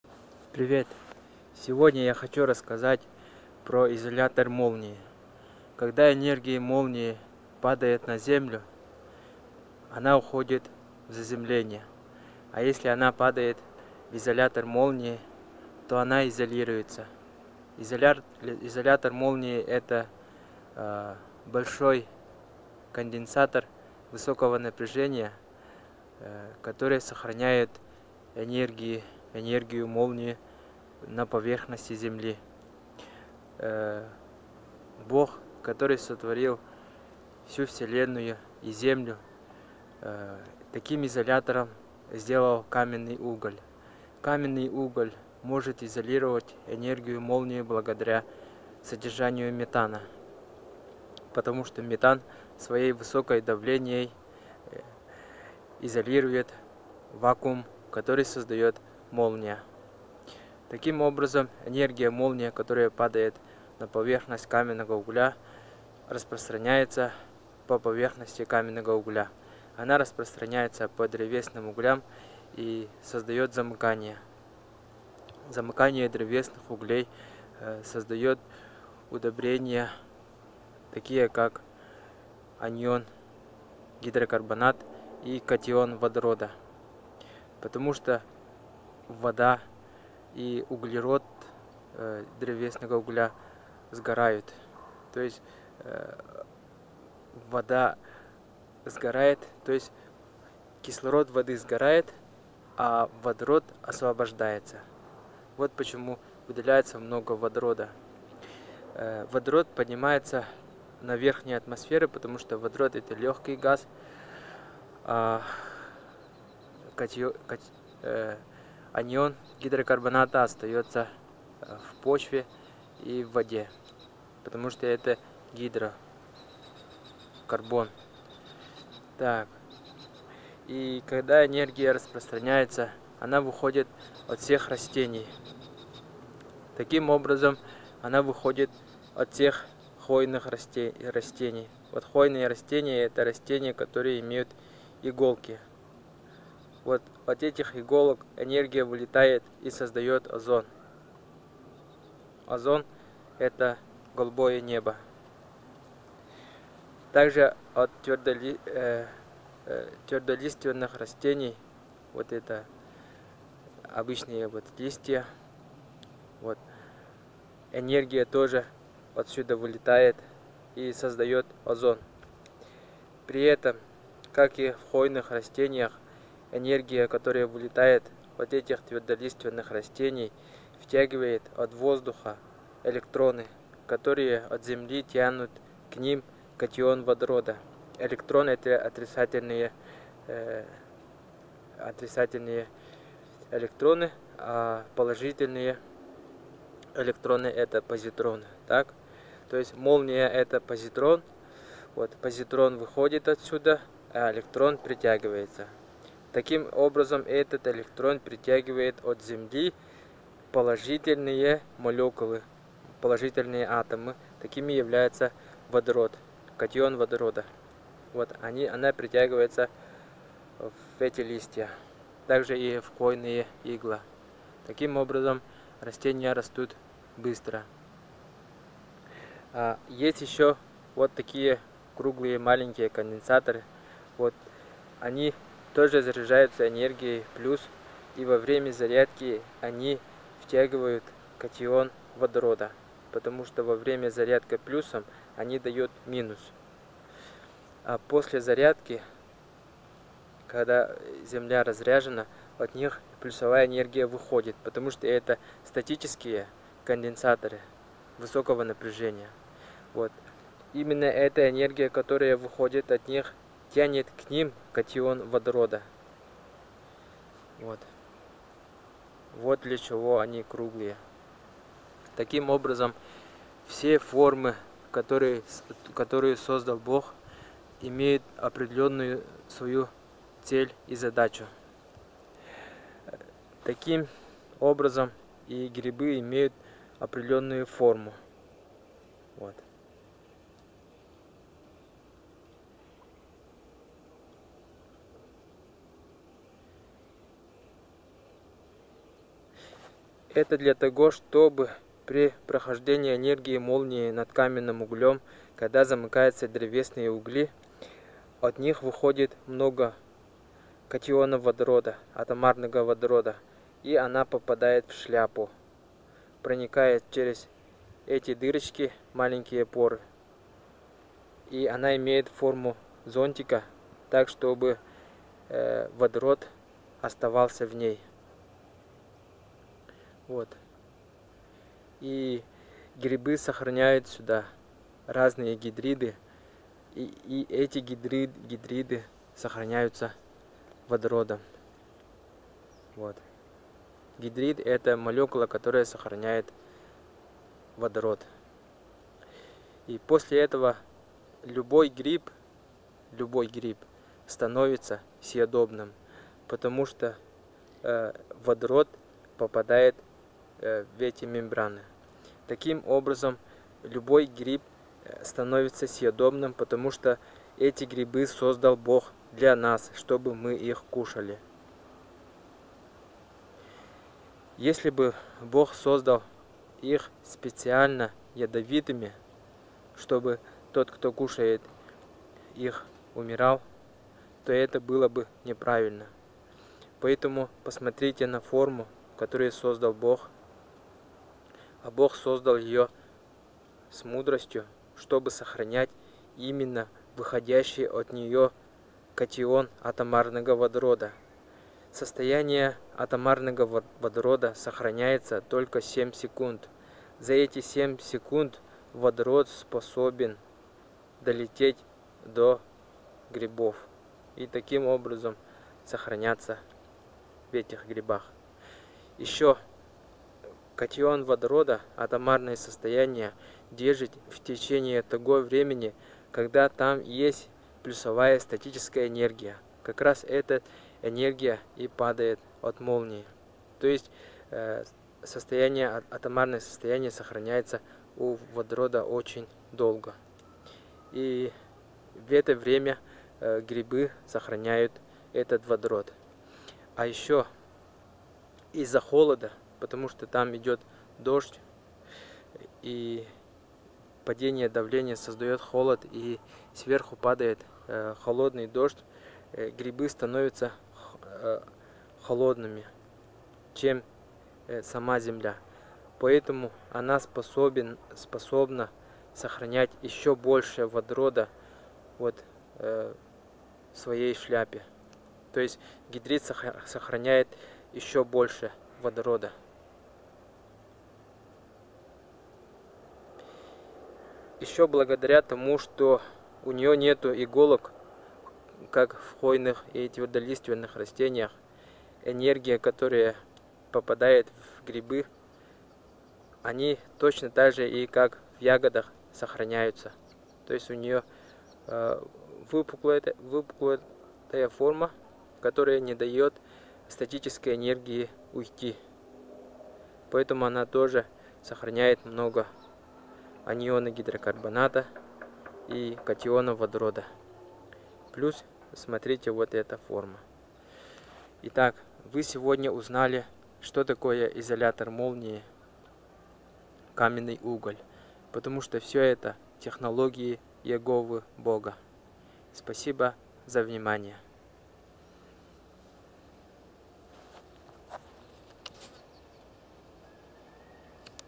🍄 Mushrooms are the technologies of Jehovah God. Science must know the technology of Jehovah.
🍄 Mushrooms. These are the technologies of Jehovah God.